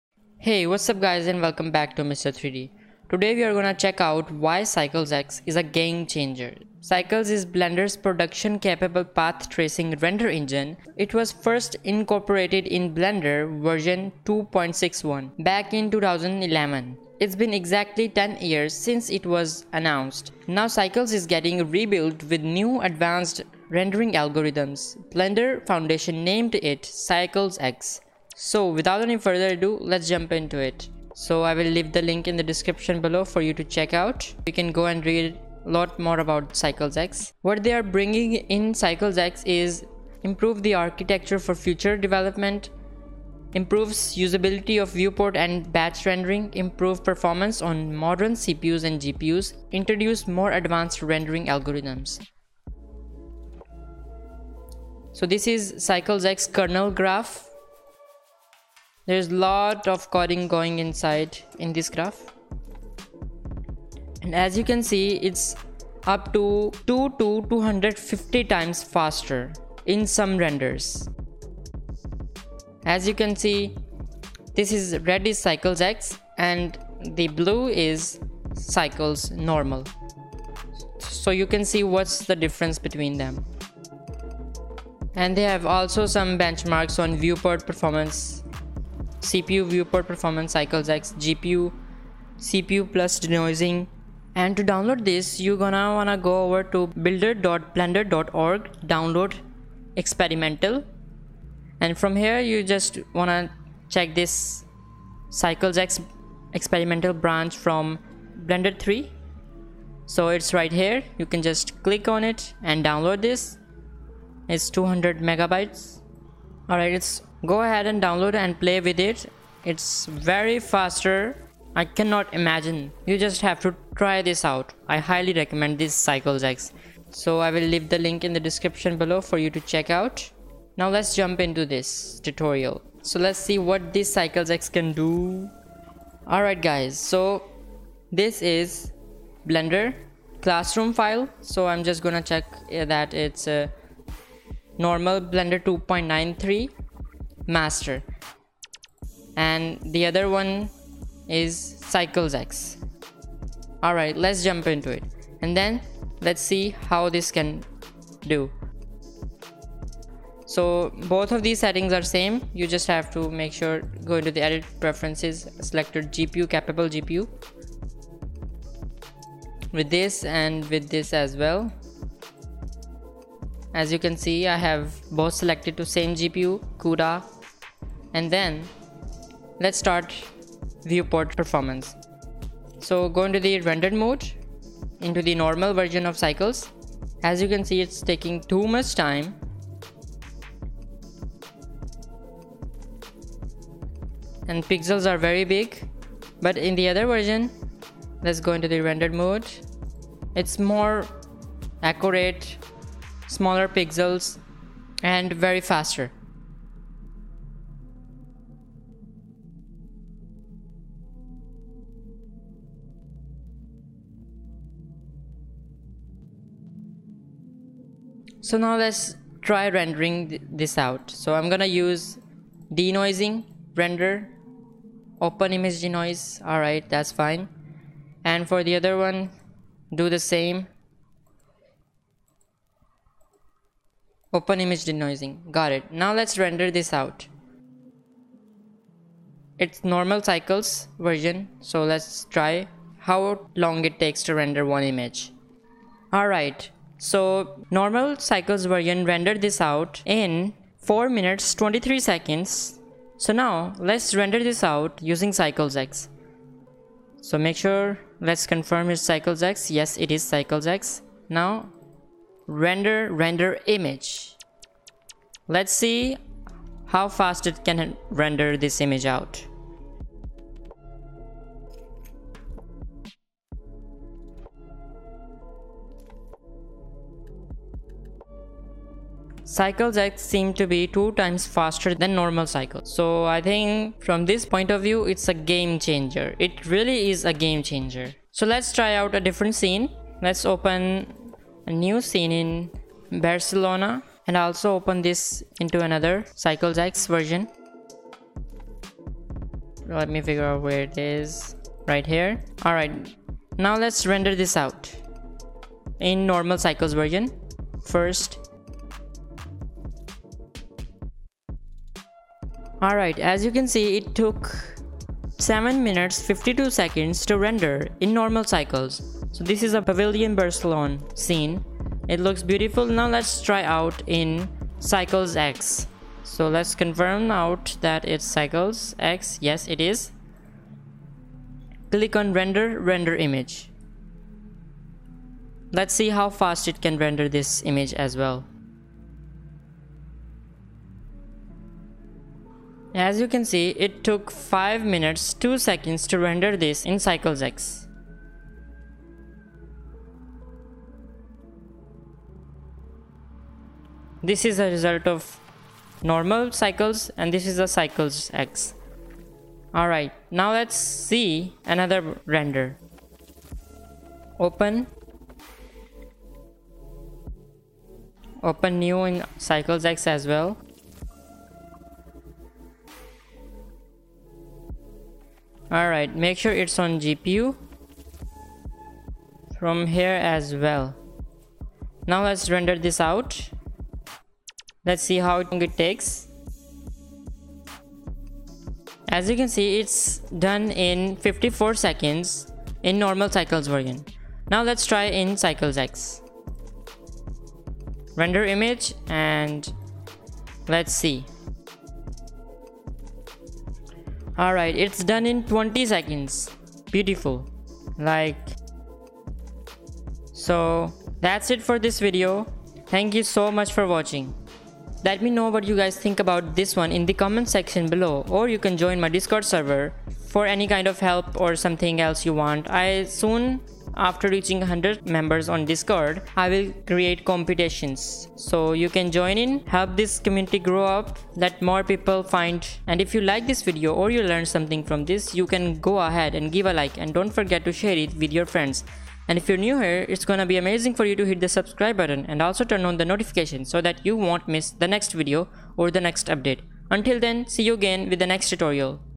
Why Cycles X is a Game Changer | Blender 3.0
Hey what's up guys and welcome back to Mr 3D today we are gone check out Why cycles x is a game changer. Cycles is Blender’s production-capable path-tracing render engine, first incorporated into release 2.61, back in 2011. it’s been exactly 10 years since Cycles was announced. Now Cycles is Getting Rebuild with New advanced rendering algorithms. Bledner Foundation named it Cycles X

Peace ✌️